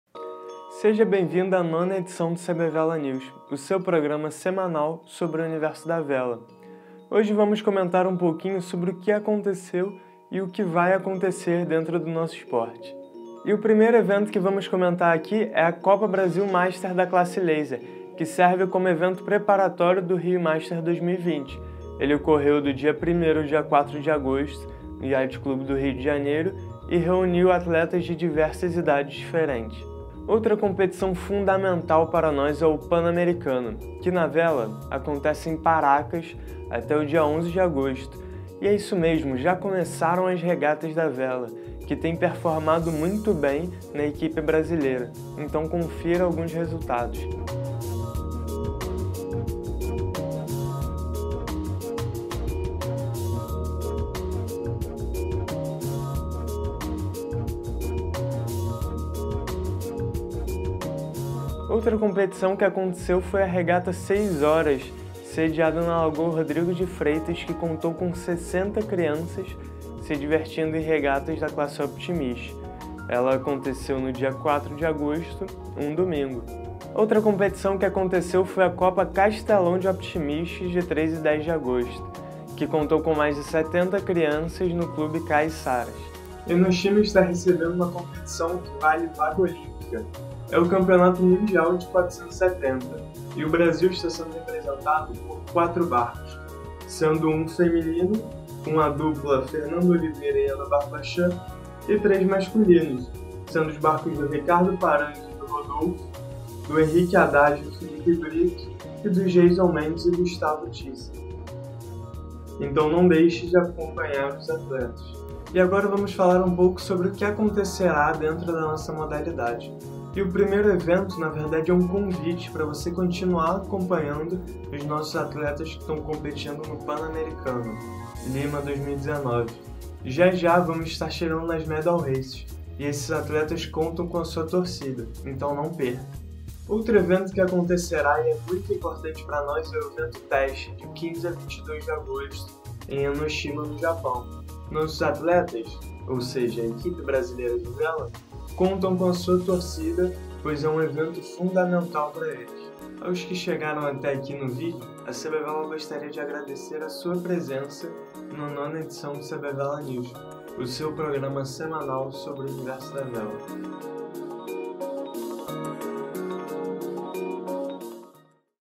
CBVela News #9 - O que está acontecendo na Vela
A CBVela, com o suporte da Know-How Learning, está trazendo mais um episódio do CBVela News para você! Nessa edição falamos um pouco do que aconteceu e o que acontecerá na nossa modalidade.